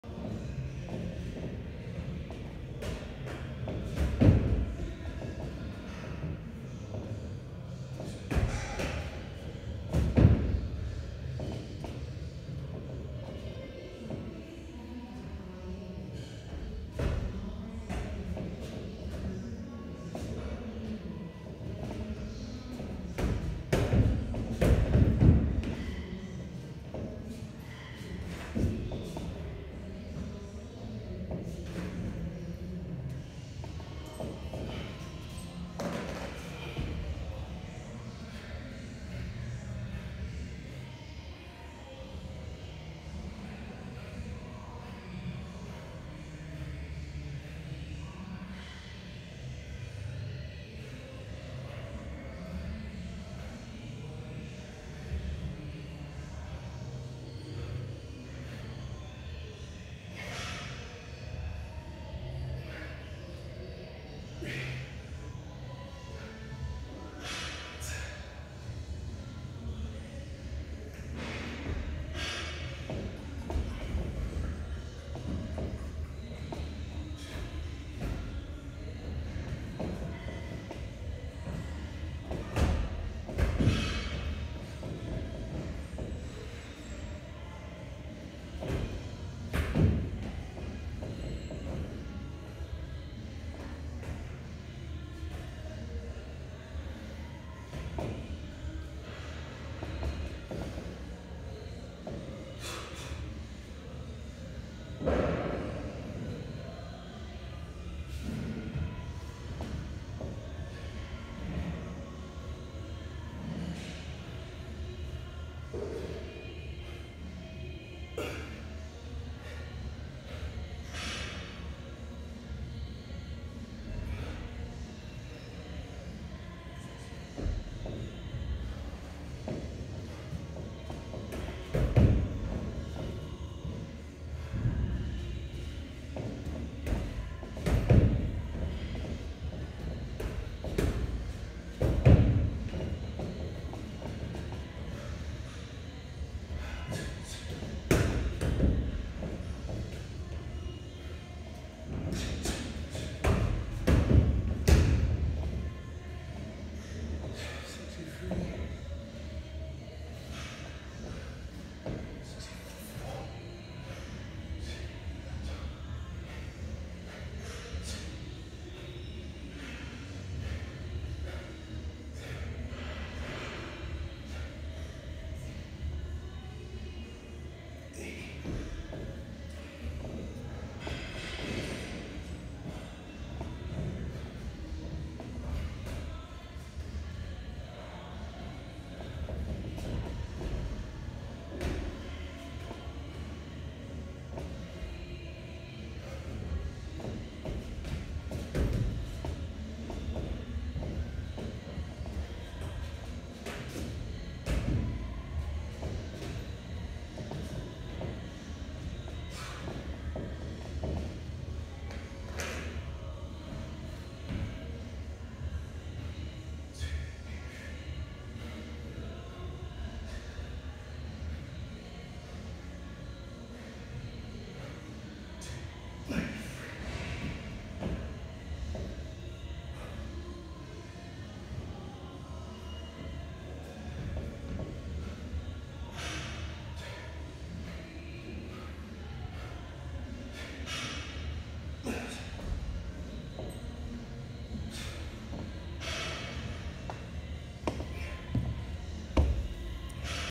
Working on Flexibility and Kicking Range in Between Pushup Sets 🦿 (100 PUSHUPS EVERY DAY — Nov. 21)
I've had an annoying shoulder issue for the past few years. No treatment has fixed it. So I'm forcing my shoulder to deal with 100 pushups of daily stress to get stronger by adaptation.